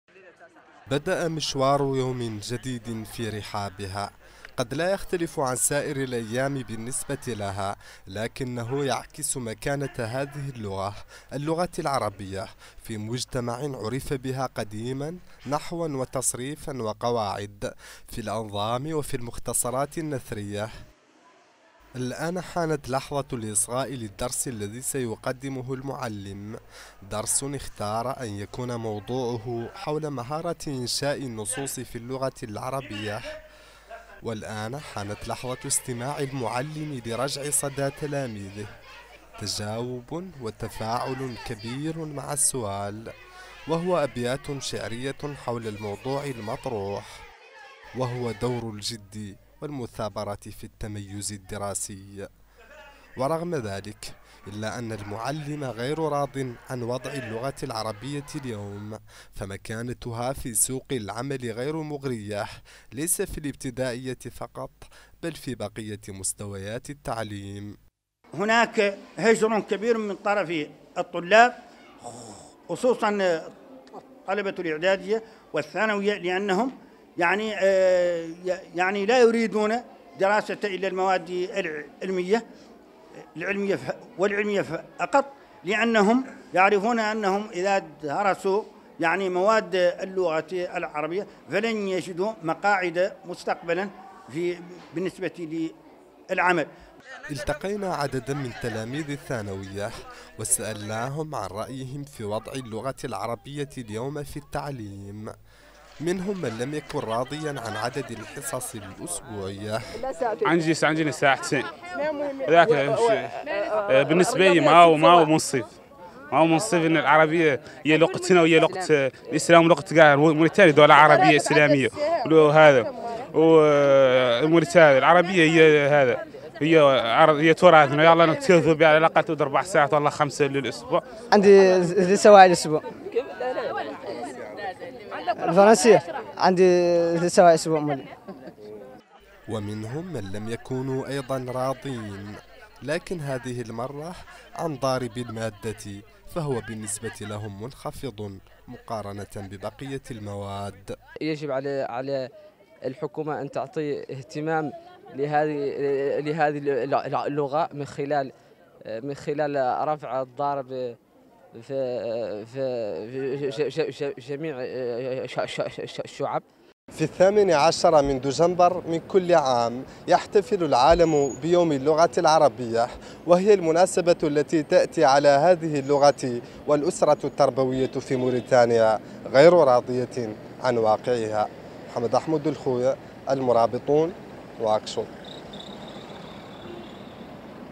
حضور اللغة العربية في التعليم بموريتانيا تقرير محمد الأمين ولد امعييف.
يحتفي العالم العربي في الثامن عشر دجمبر، باليوم العالمي للغة العربية، وفي موريتانيا تحتل اللغة العربية مكانة هامة، حيث تعتبر لغة التدريس الأولى، رغم الحديث عن تراجع الاهتمام بها.
ويرى البعض أن اللغة العربية، التي كثيرا ما اشتهرت بها موريتانيا، وحملت لقب بلاد المليون شاعر، يرى أنها اليوم ليست مغرية في سوق العمل بالبلاد.
تقرير: محمد محمود ولد خوي.